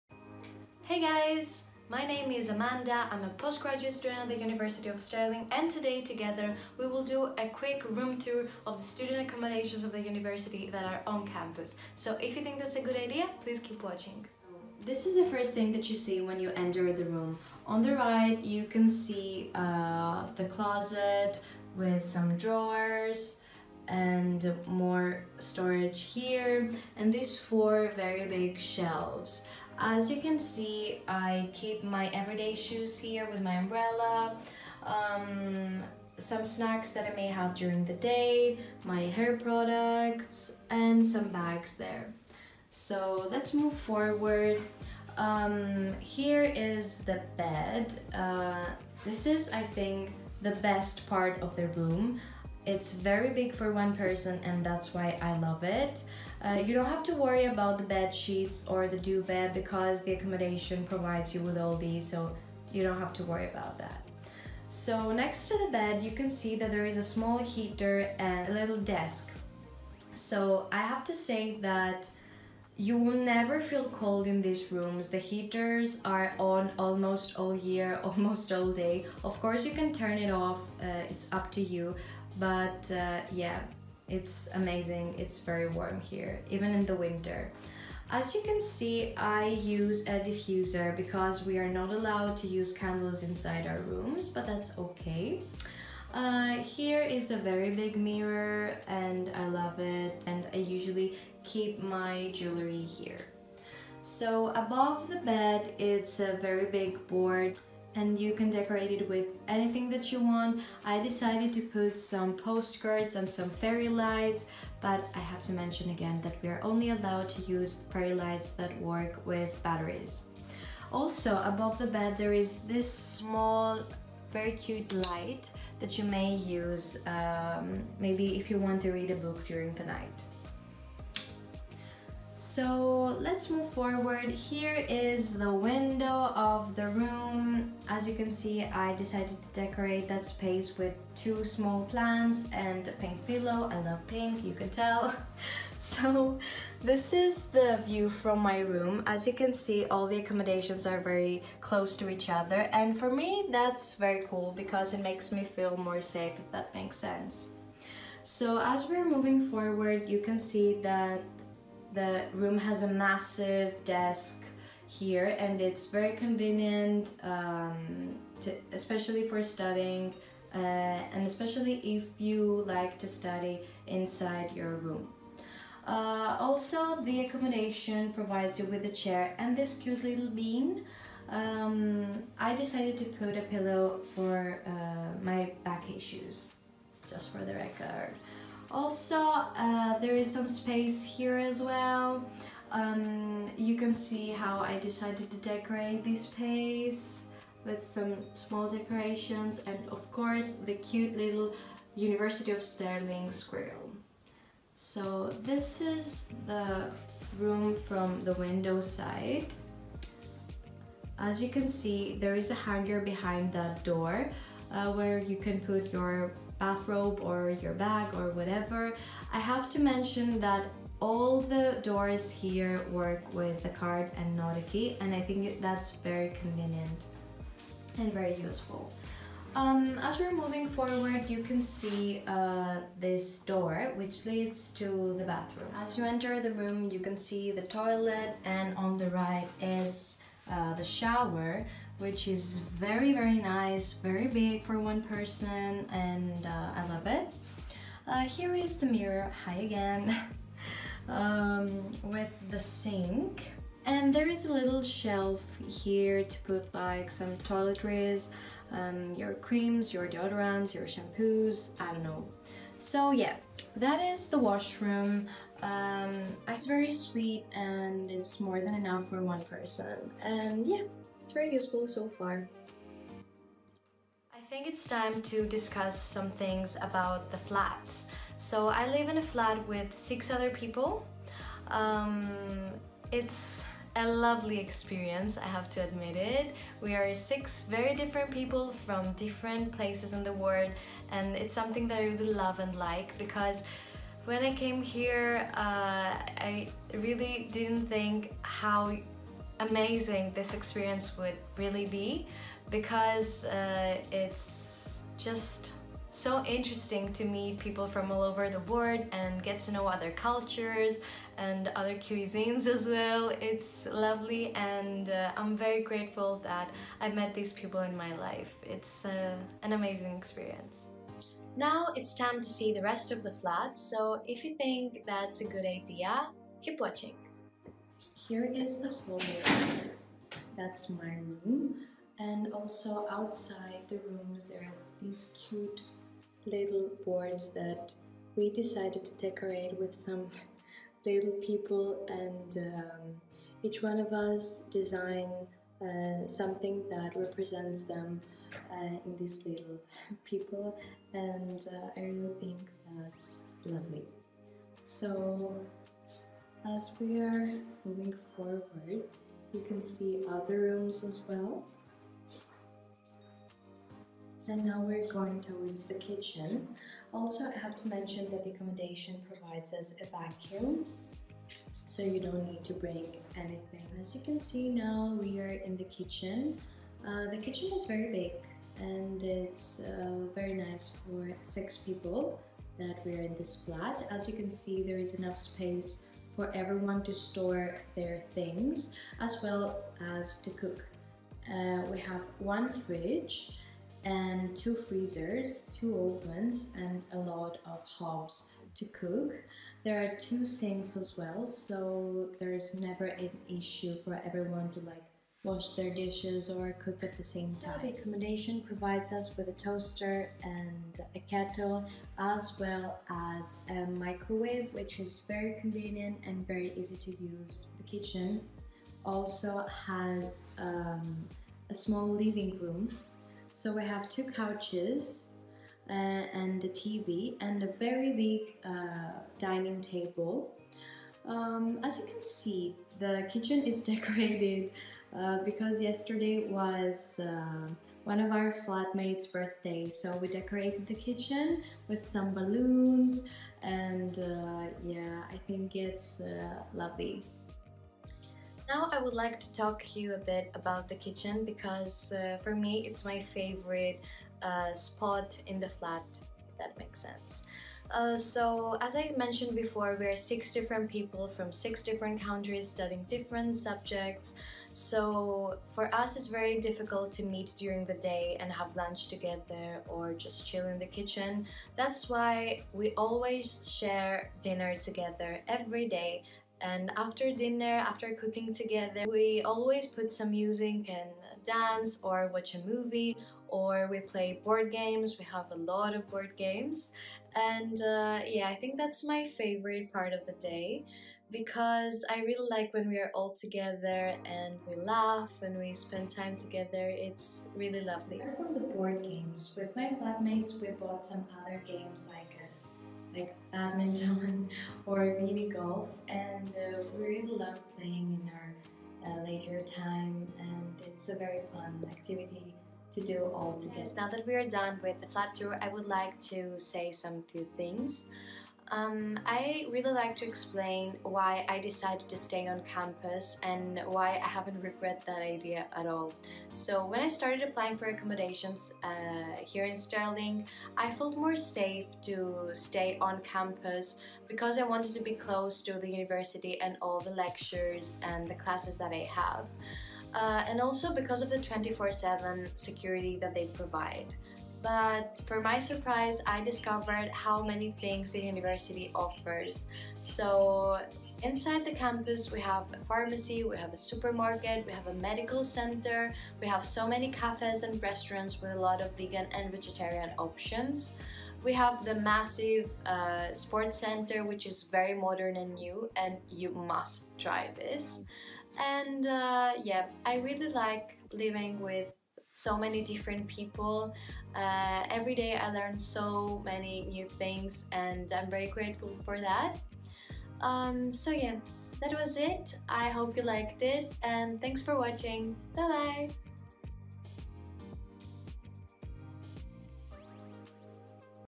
University Room Tour - Beech Court University of Stirling *student vlog*
Check out UofStirling vlogger Amanda's latest video where she takes us through her on-campus accommodation with a personal guided tour!

Want to know more about accommodation at Stirling?

0:00 Intro
0:14 Beech Court Room Tour
3:50 Ensuite bathroom
4:34 Living in Beech Court accommodation (my experience)
5:21 Tour of the communal areas
7:28 Making friends (my favourite part of student halls)
8:35 Why I decided to stay on campus in student accommodation